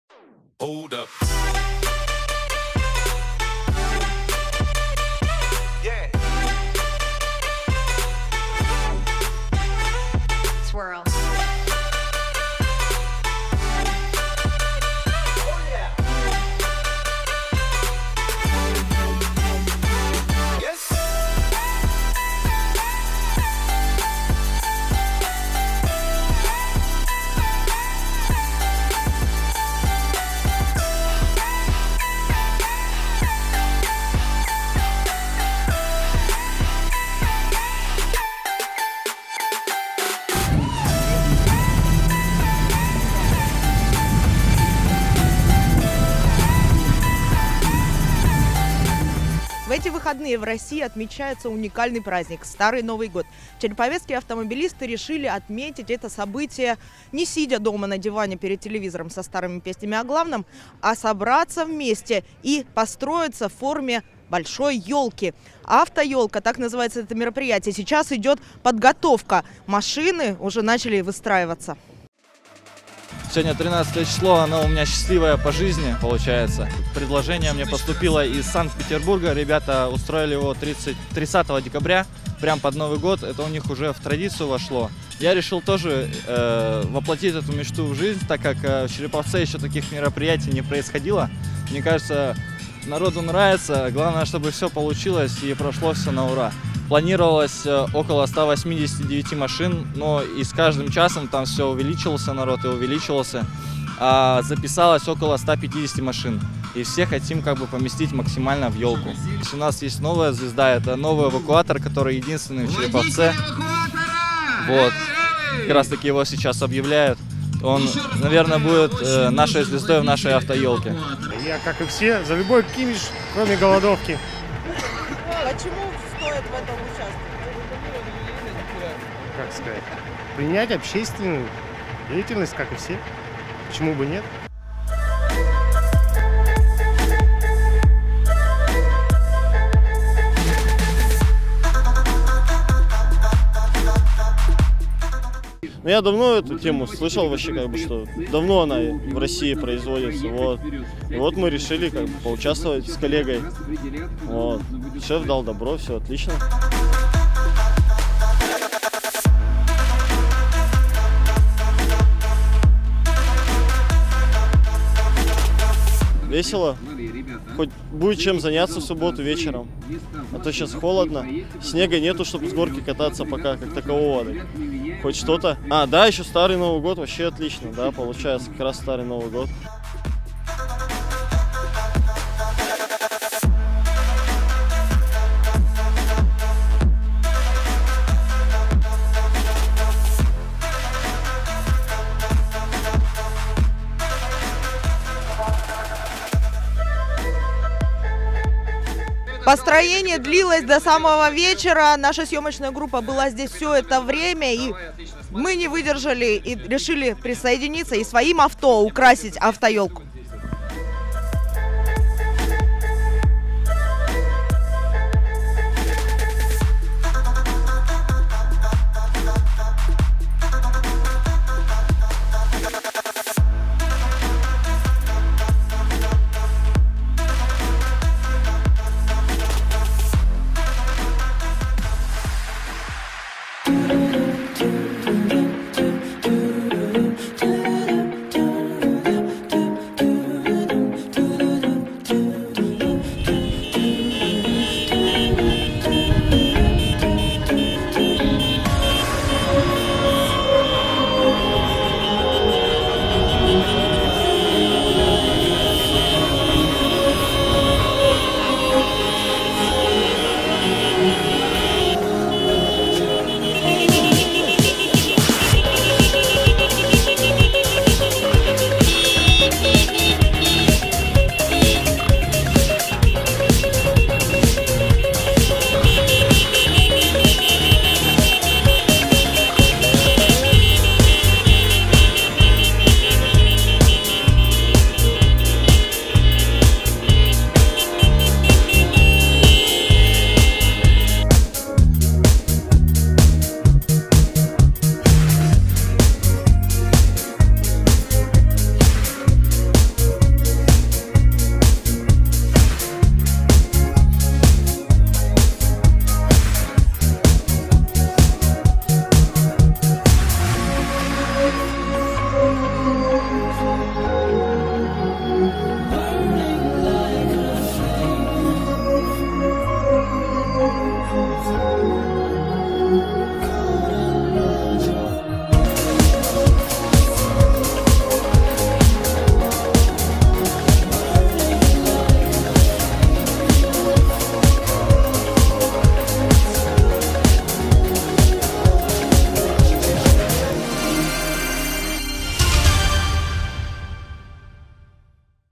Ёлку из более чем 150 машин создали череповецкие автолюбители
Флешмоб «Авто-ёлка» состоялся 13 января в канун Старого Нового Года у д. Борисово Череповецкого района. В форму масштабной ёлки выстроились более 150 автомобилей, в том числе 2 автобуса и эвакуатор. В городе металлургов подобная затея воплощается впервые, хотя такие флешмобы по России уже не редкость. Последняя авто-ёлка появилась под Новый Год в Санкт-Петербурге.